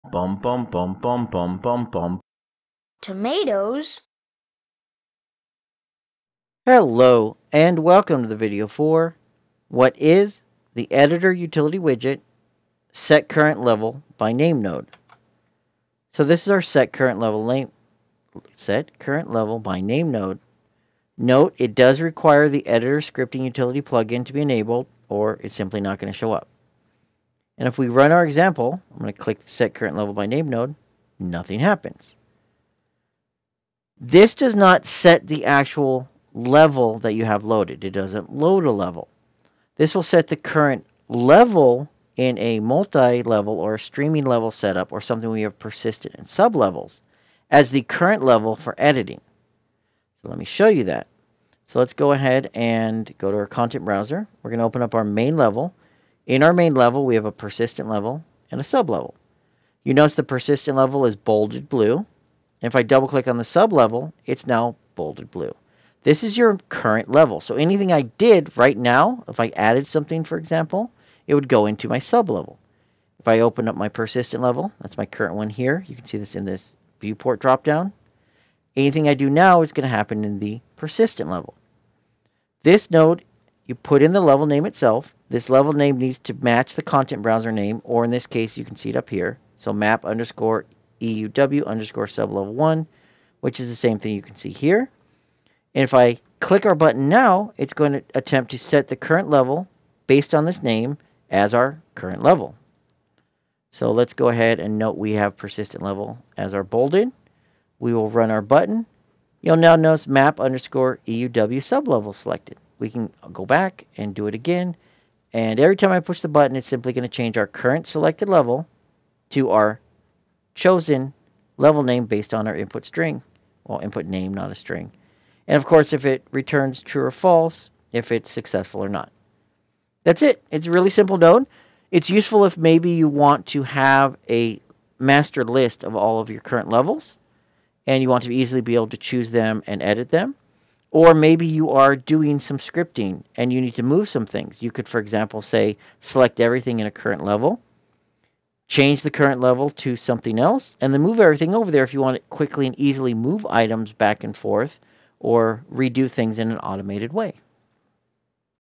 WTF Is? EUW - Set Current Level By Name in Unreal Engine 4 ( UE4 )
What is the Set Current Level By Name node for Editor Utility Widgets in Unreal Engine 4